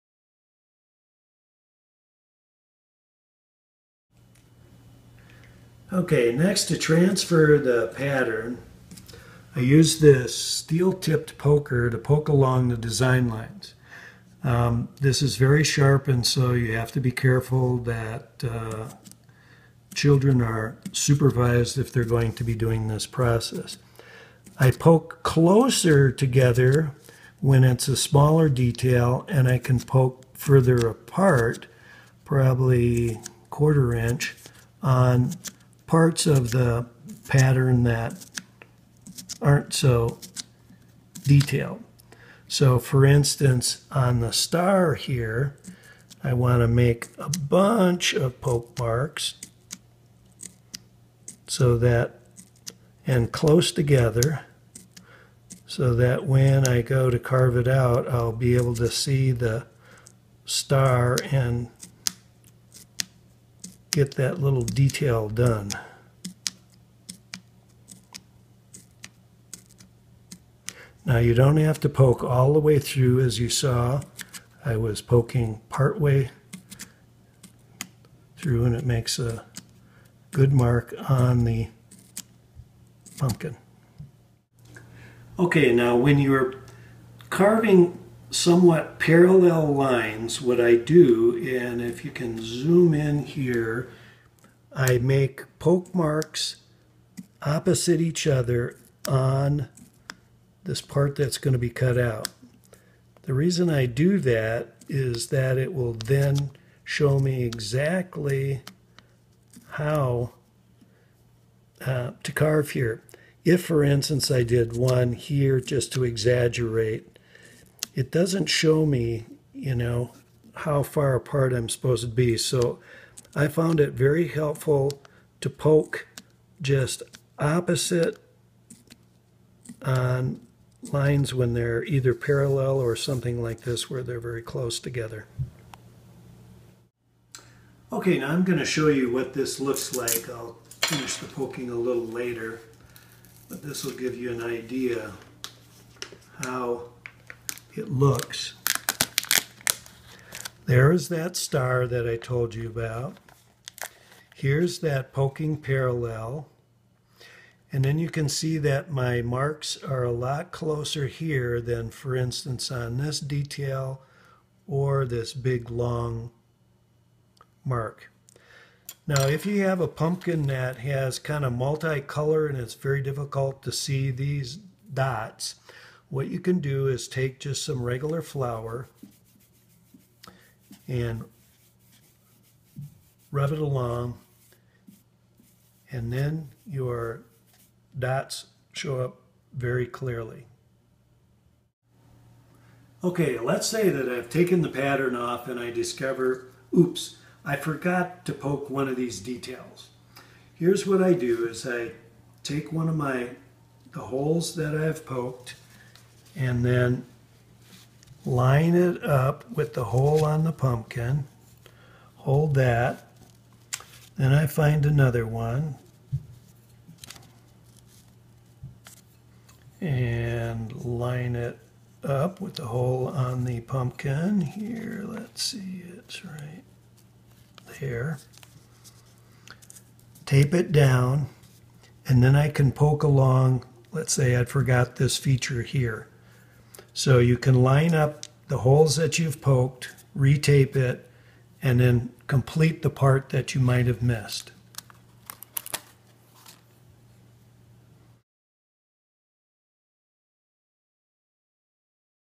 Transferring Your Pattern to Your Pumpkin
Master carver John "Grampa" Bardeen shows you how to transfer your pattern to your pumpkin using a steel-tipped poker. Grampa Bardeen's Family Pumpkin Carving Sets are available at GrampaBardeen.com and Amazon.com (eligible for Prime shipping).

for more pumpkin carving tips and tricks!